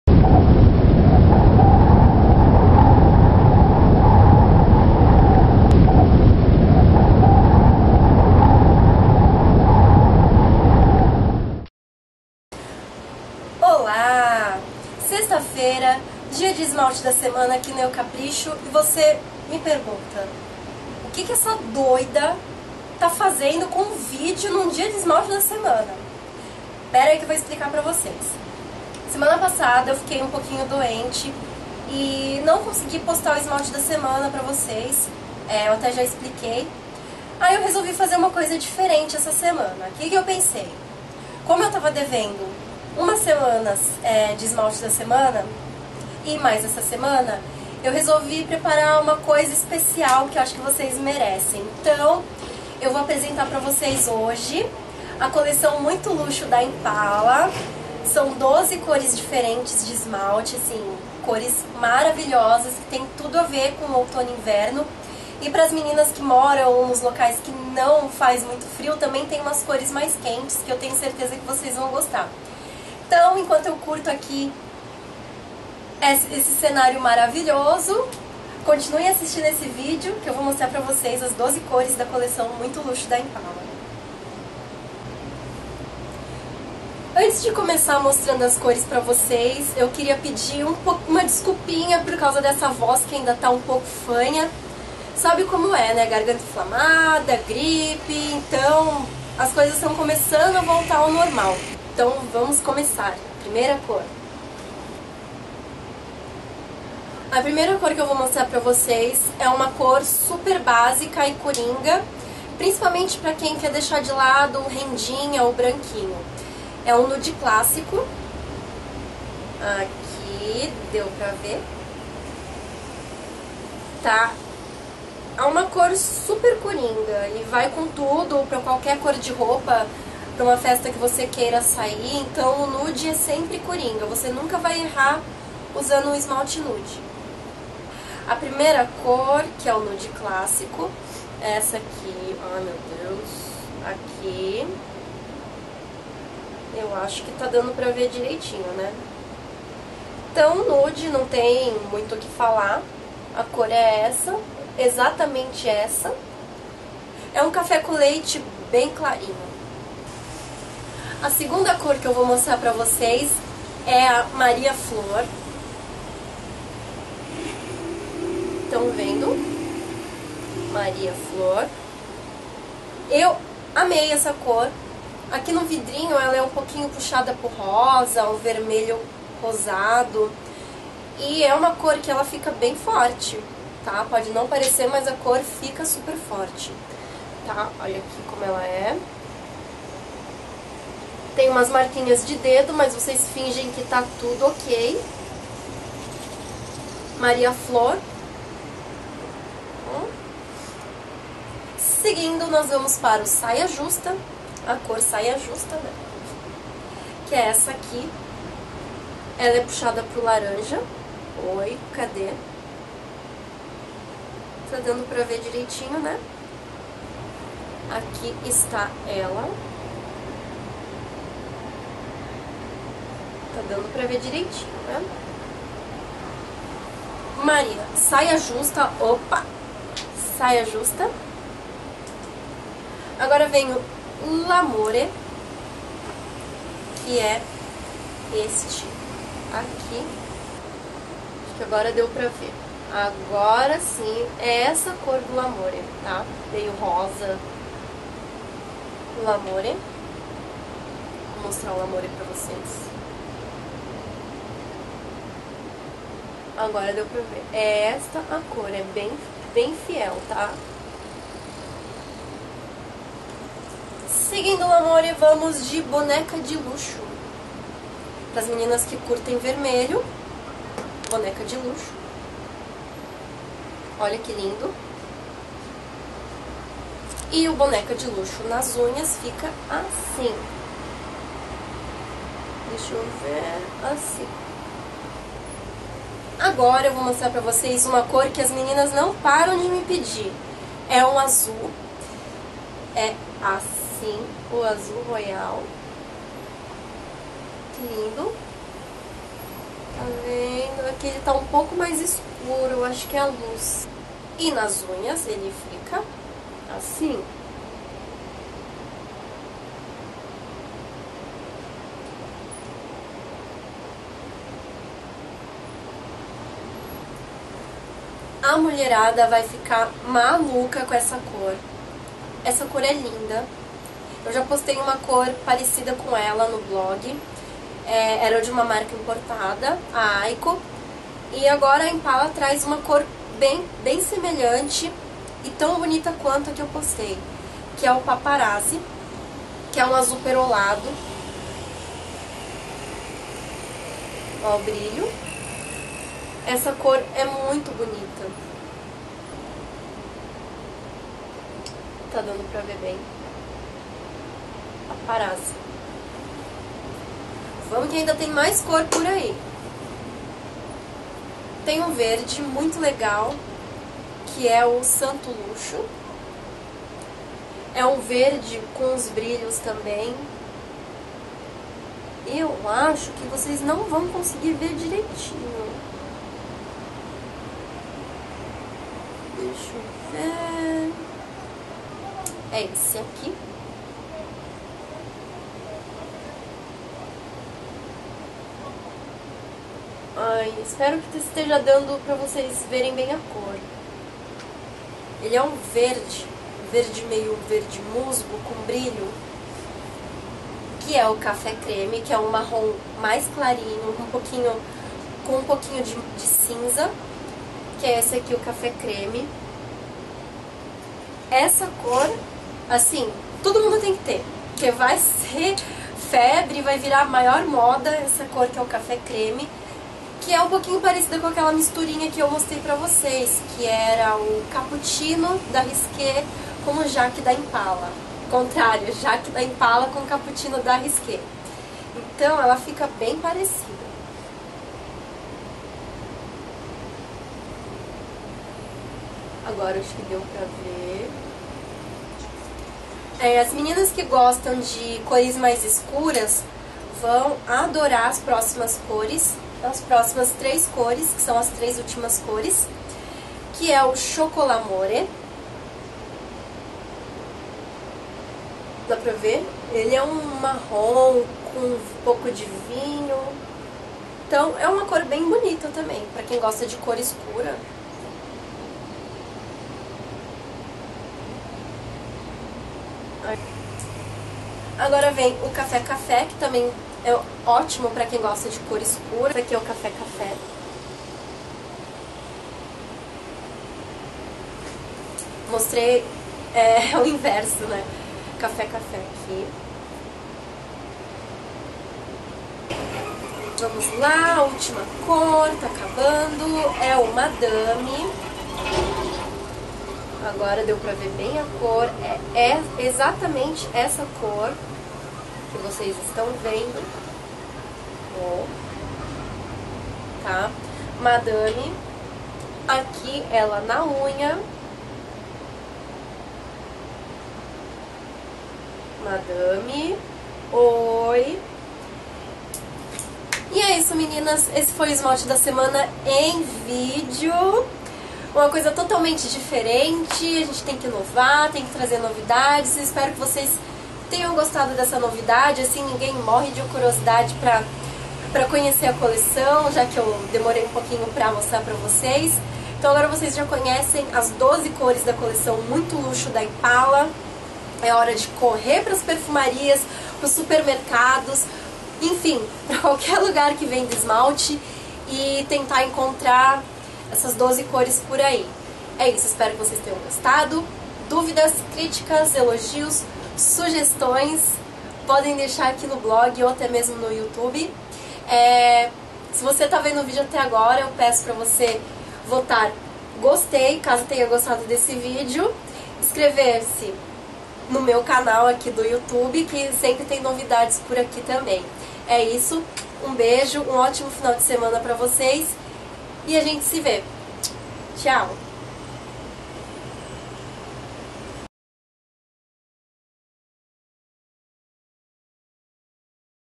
Esmalte da Semana - Coleção Muito Luxo - Impala
Neste vídeo vou mostrar para vocês os 12 lançamentos em esmalte da Impala. Todos os detalhes da coleção Muito Luxo.